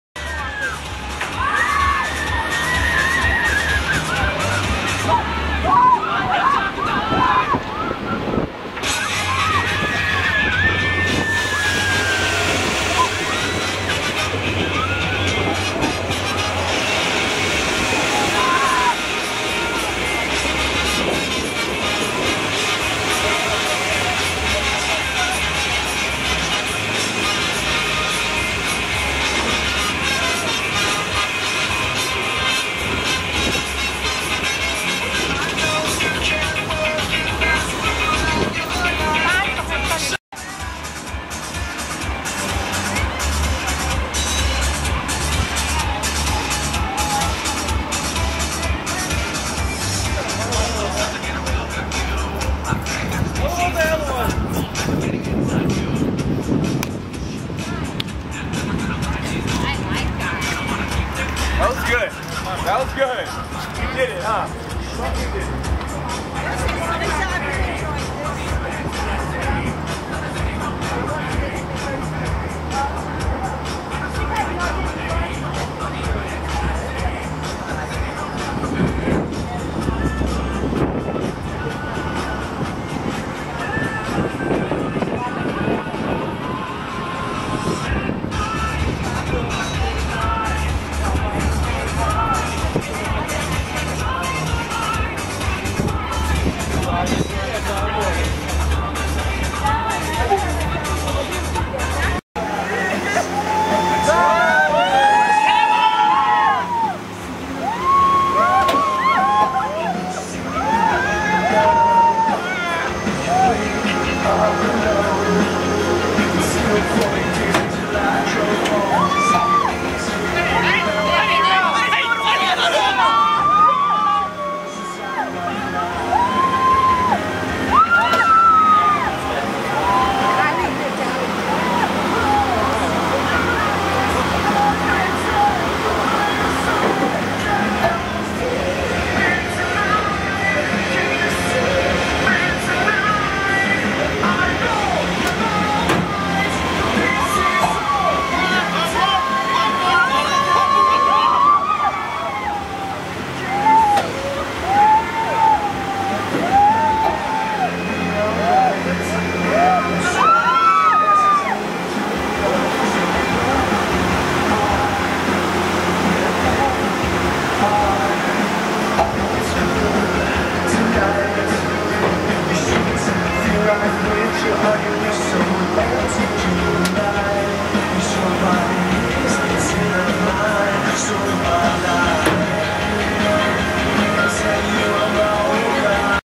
Stratosphere 109th Floor View in Las Vegas
Stratosphere Casino Tower's  109th Floor view of Las Vegas and all three rides on top of this tower.